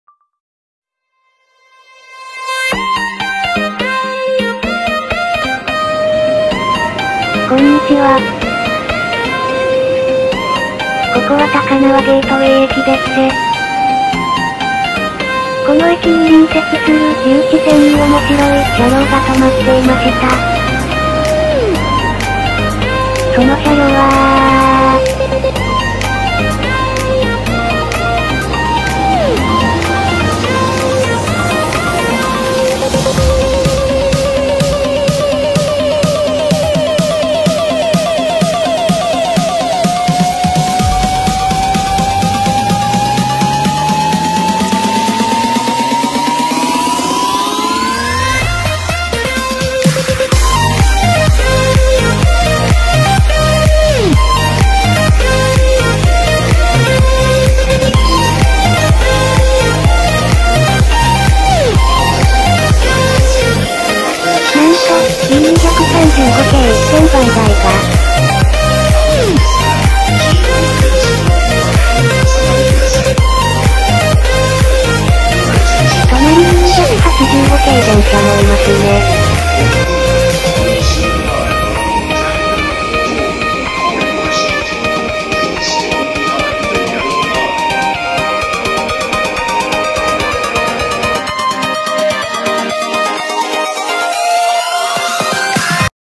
留置線にレアな車両が....
高輪ゲートウェイ駅に隣接する東京総合車両センターの留置線になかなかレアな列車が止まっていました。